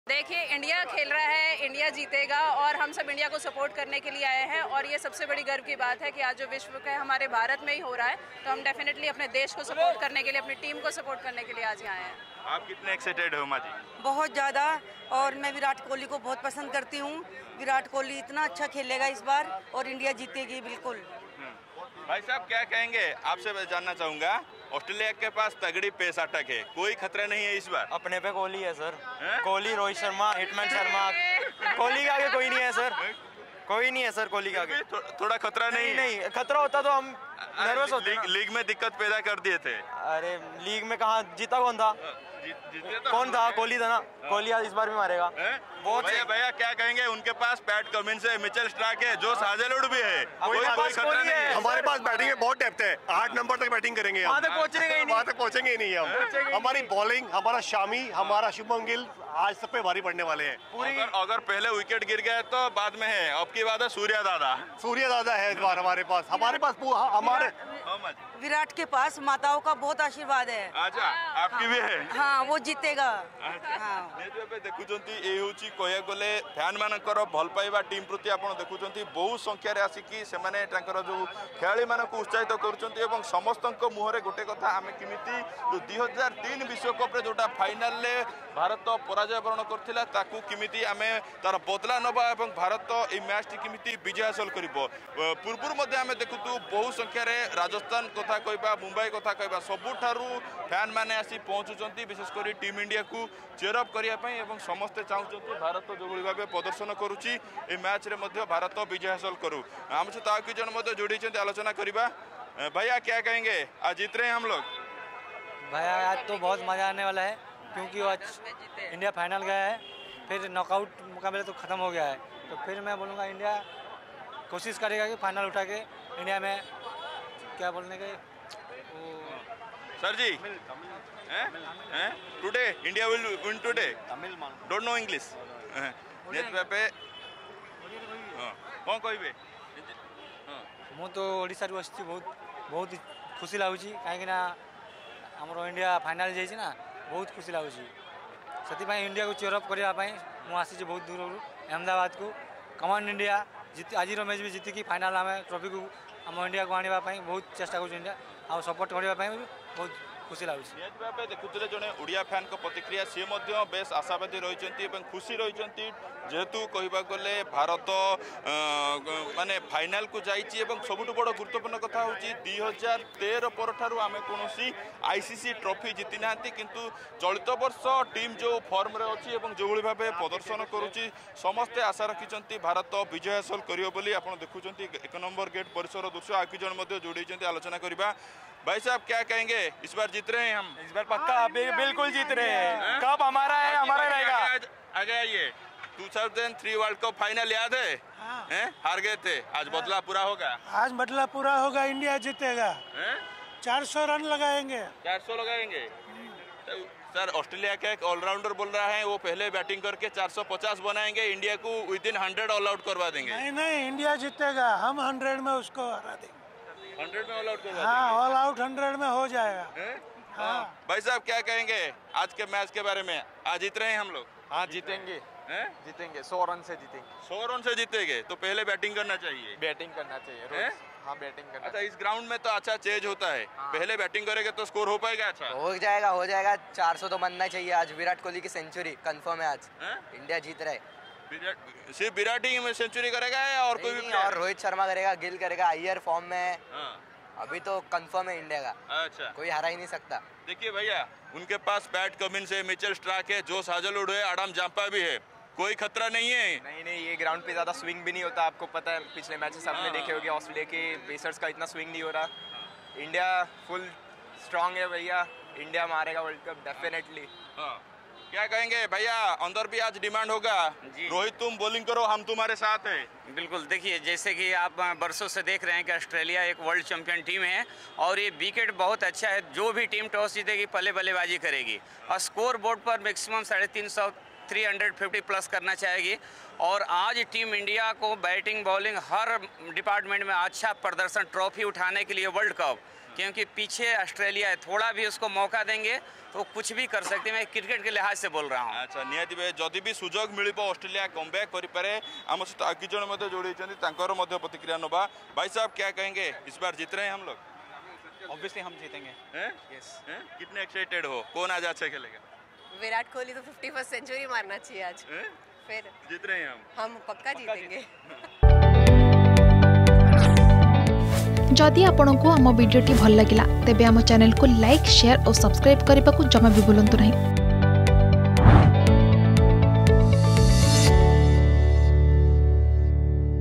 ଅଷ୍ଟ୍ରେଲିଆ ବୋଲରଙ୍କ ଉପରେ କୋହଲି ଭାରି ପଡ଼ିବେ || World Cup Final || India vs Australia
ଅଷ୍ଟ୍ରେଲିଆ ବୋଲରଙ୍କ ଉପରେ କୋହଲି ଭାରି ପଡ଼ିବେ: ଟିମ୍ ଇଣ୍ଡିଆ ଫ୍ୟାନ ।